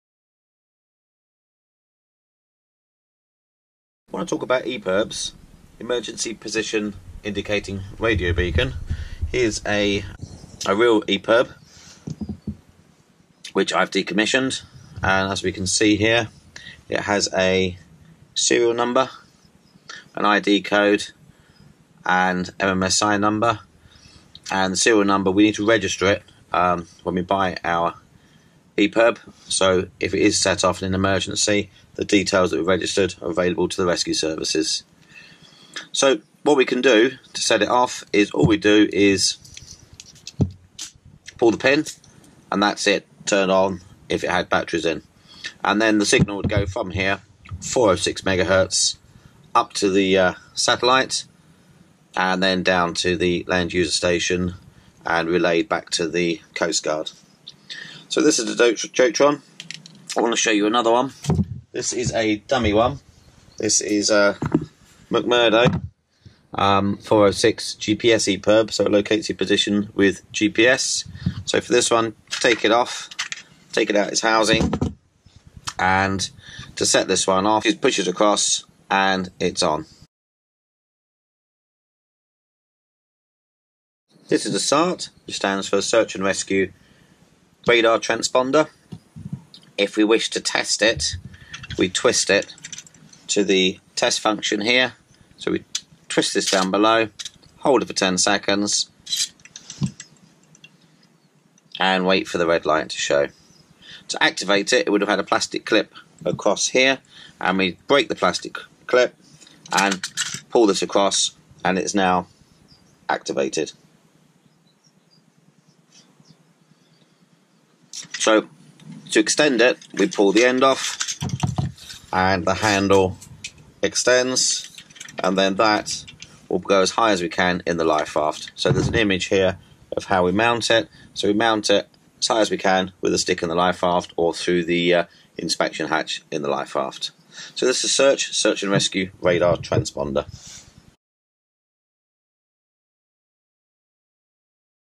EPIRB and SART use
Part of the City Sailing Sea Survival and Offshore Safety Course, we show you and explain both the EPIRB and the SART, this is part of the City Sailing RYA Course